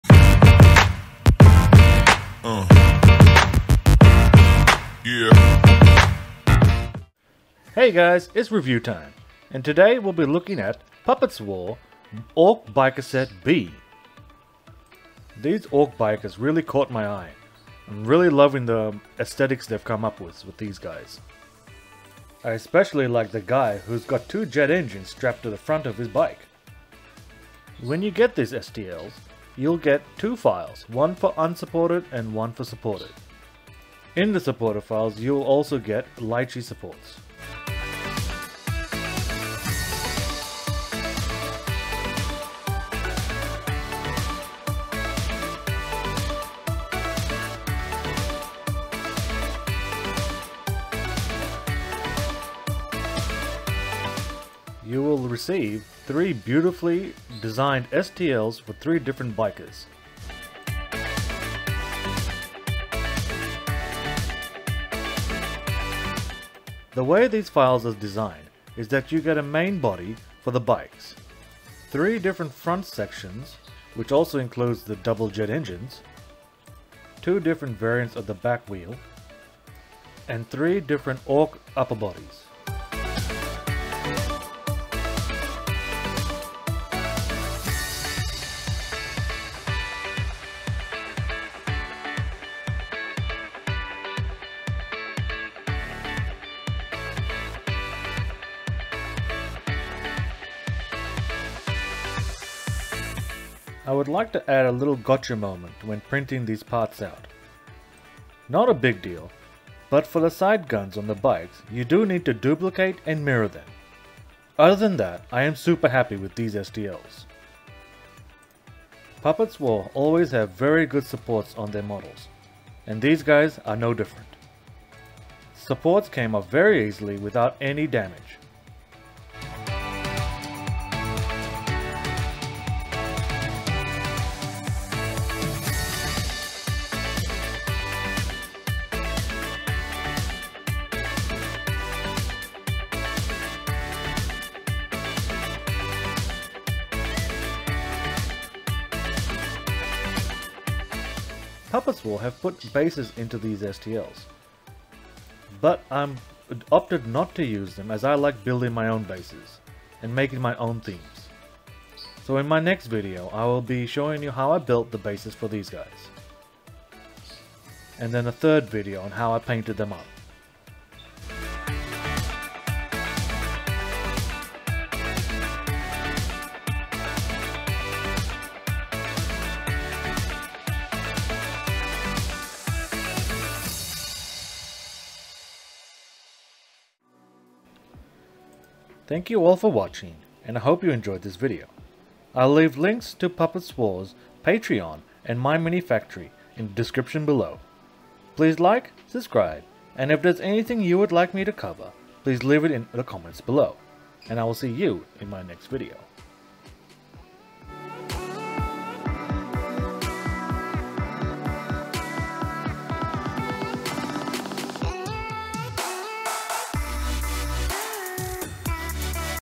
3D Printed Orc Bikers From Puppetswar Review
Today I'm going to be reviewing the Orc Bikers set B from Puppetswar. Enjoy.